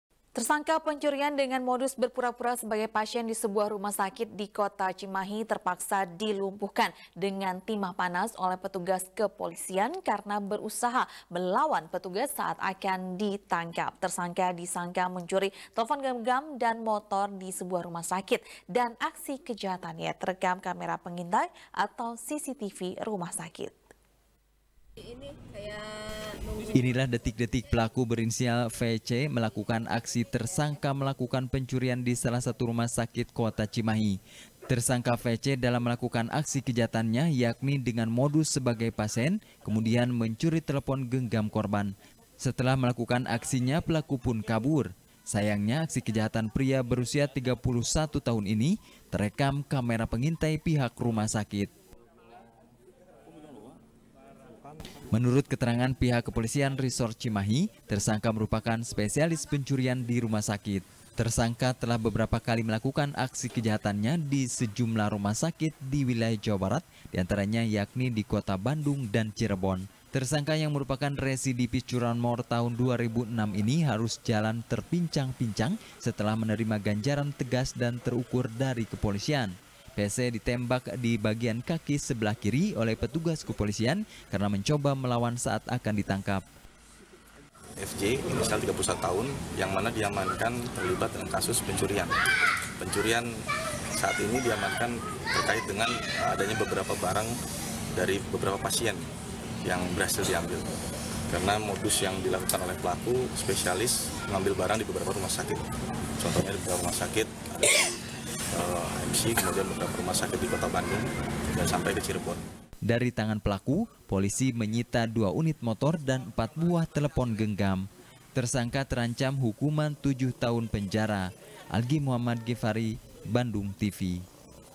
Aksi Pencurian Di Rumah Sakit Terekam Kamera Pengintai | SBR | BANDUNGTV
SBR | BANDUNGTV | KRISANTY | Tersangka pencurian,dengan modus berpura-pura  sebagai pasien di sebuah rumah sakit di kota cimahi,terpaksa dilumpuhkan dengan timah panas oleh petugas kepolisian,karena berusaha melawan petugas saat akan ditangkap.tersangka,disangka mencuri telepon genggam dan motor,di sebuah rumah sakit.aksi kejahatannya terekam kamera pengintai atau cctv rumah sakit.